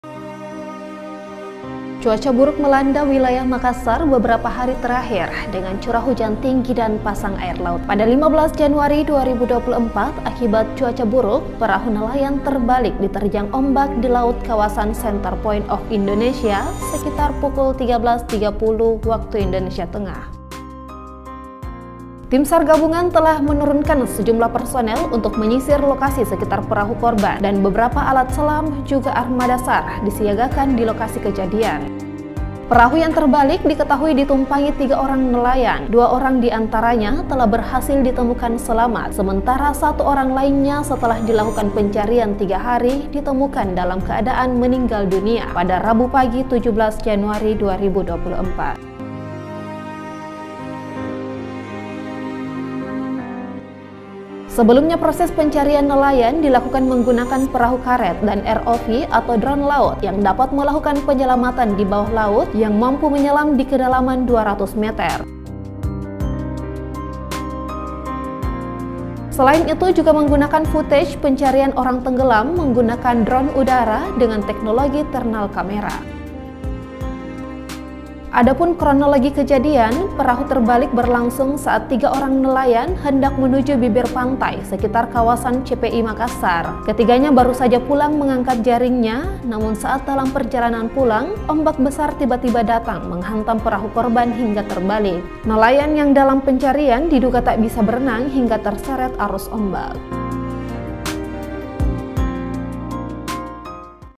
Nelayan yang Hilang di Laut CPI Makassar Ditemukan Tewas
Setelah pencarian tiga hari, Abdul (30 Tahun) berhasil ditemukan oleh Tim SAR Gabungan dalam keadaan meninggal dunia pada Rabu pagi (17/1/2024).